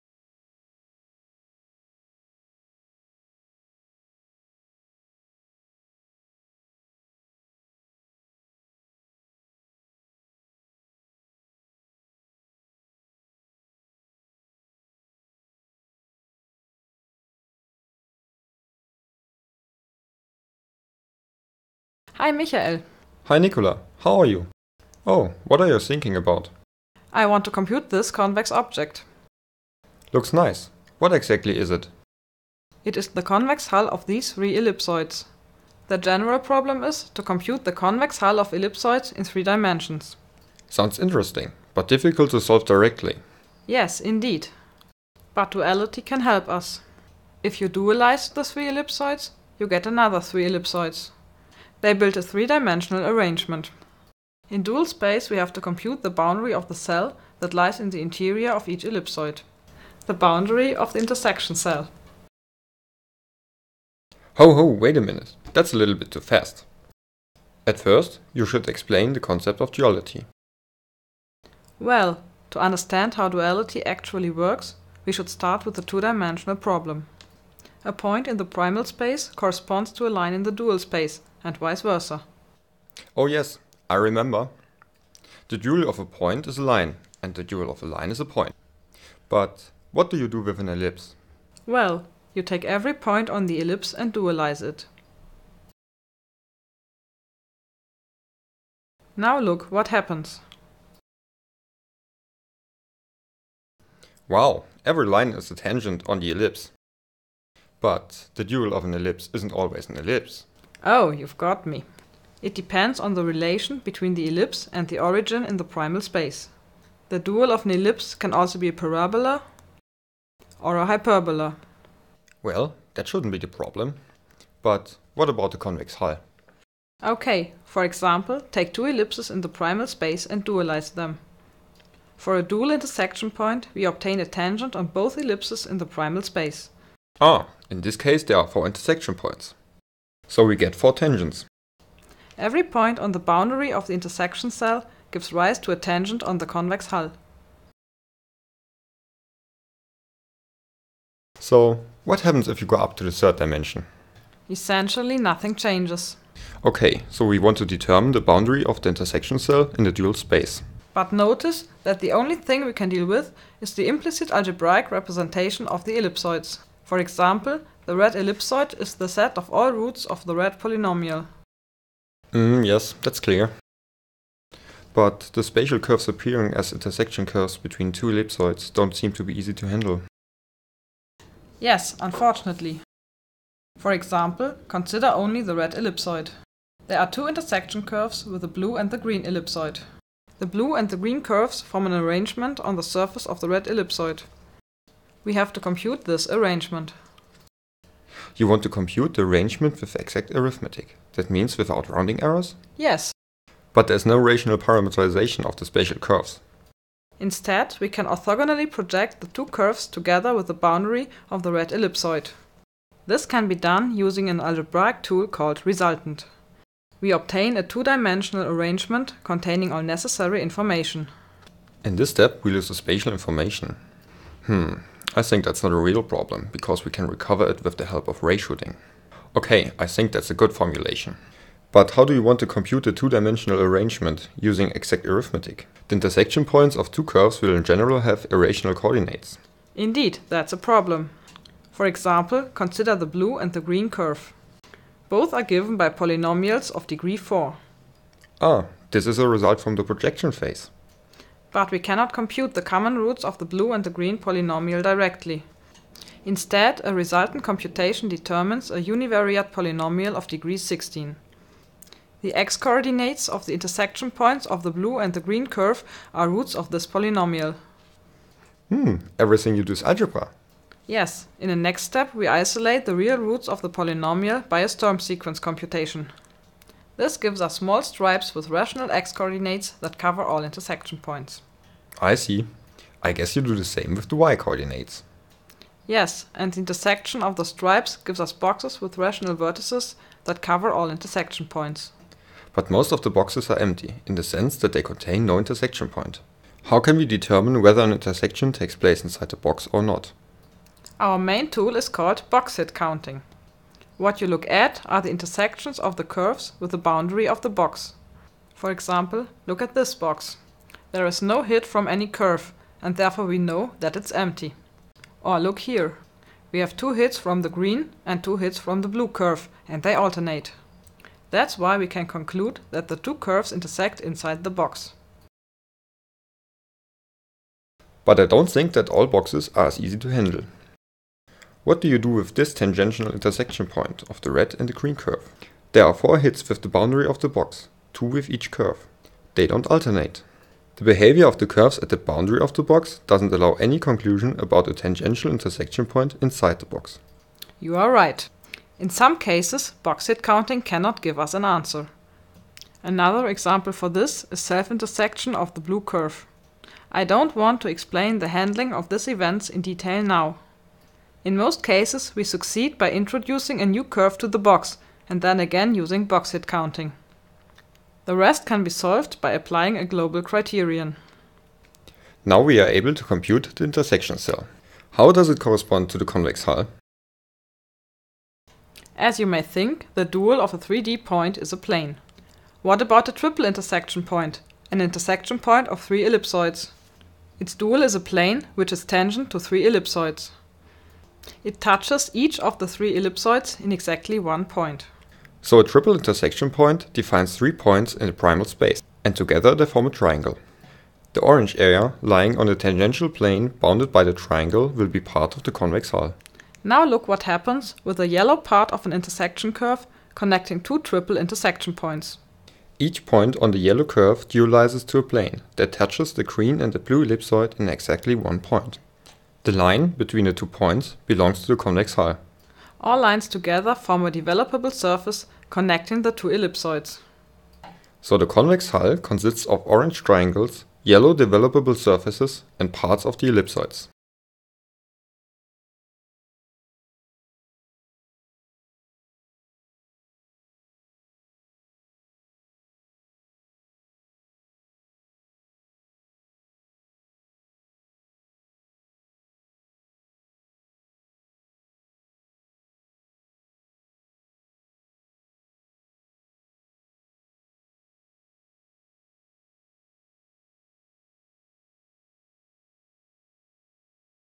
The Convex Hull of Ellipsoids SoCG 2001
SoCG Video from 2001.

The treatment of curved algebraic surfaces becomes more and more the
focus of attention in Computational Geometry. We present a video that
illustrates the computation of the convex hull of a set of
ellipsoids. The underlying algorithm is an application of our
work on determining a cell in a 3-dimensional arrangement of
quadrics, see [1]. In the video, the main
emphasis is on a simple and comprehensible visualization of the
geometric aspects of the algorithm. In addition, we give some insights
into the underlying mathematical problems.